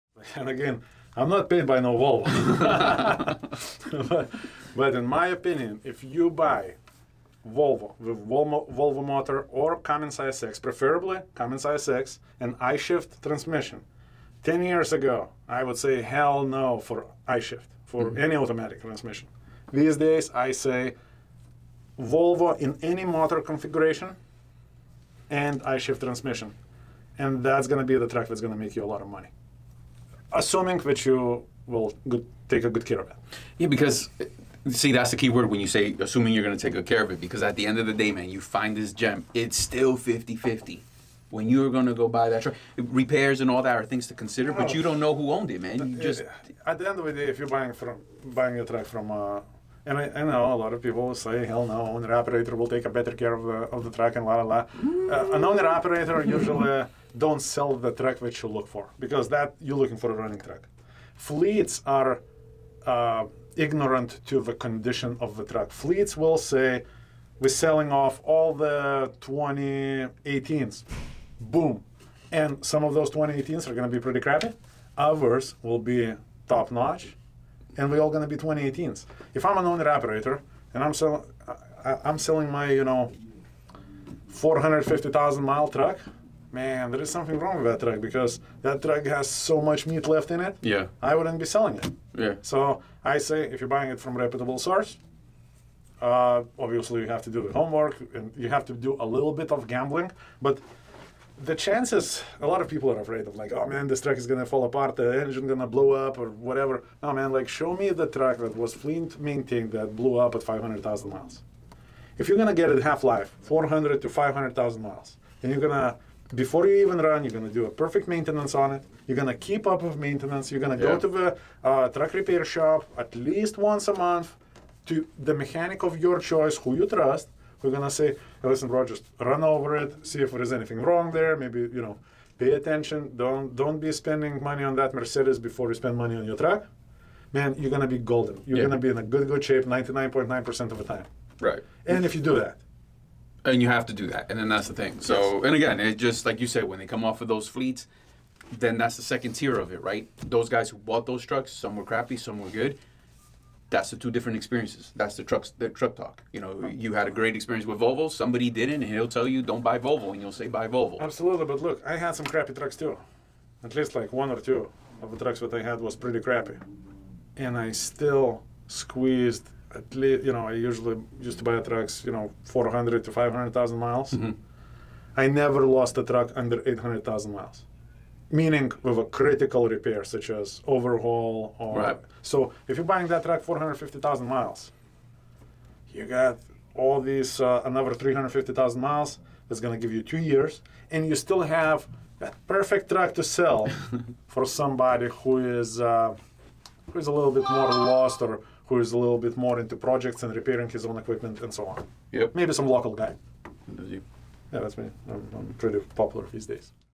Mike's opinion: Best truck for Owner-Operator.
If Mike had to do it again, what would be his truck choice?